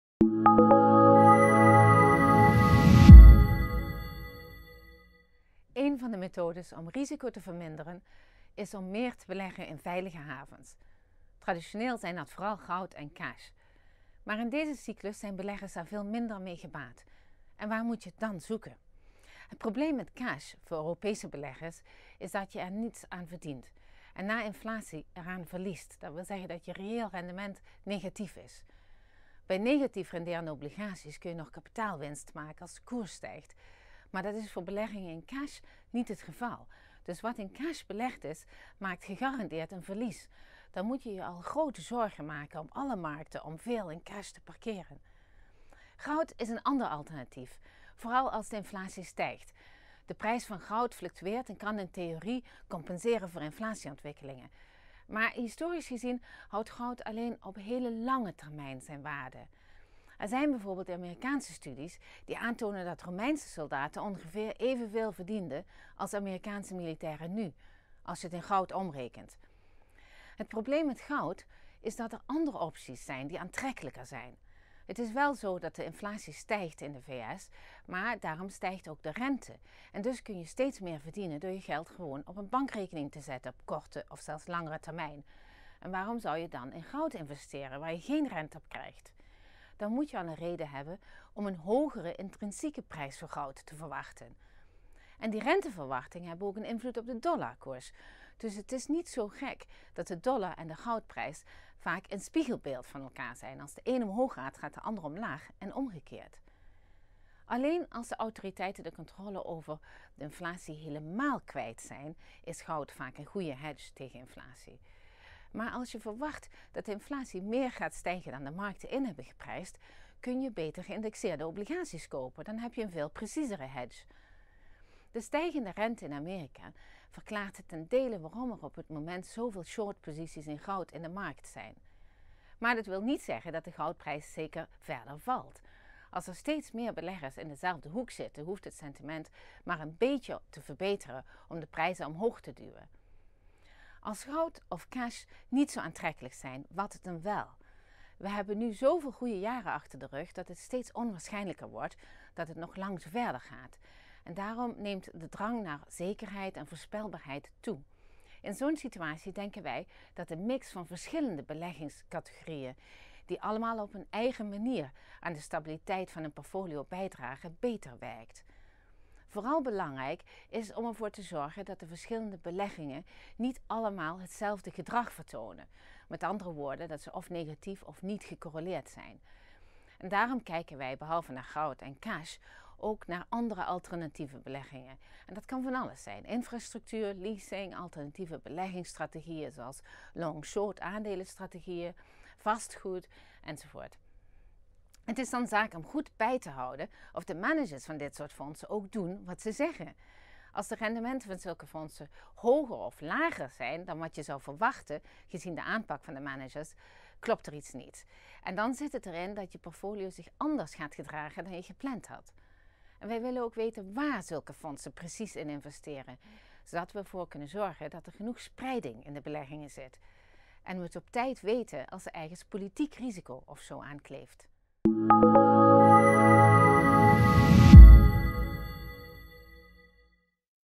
Alternatieven voor traditionele veilige havens
Een methode om risico's te verminderen is om meer te beleggen in veilige havens: traditioneel goud en cash. Europe Editor Grethe Schepers van Fidelity International onderzoekt wat aantrekkelijkere opties zouden kunnen zijn.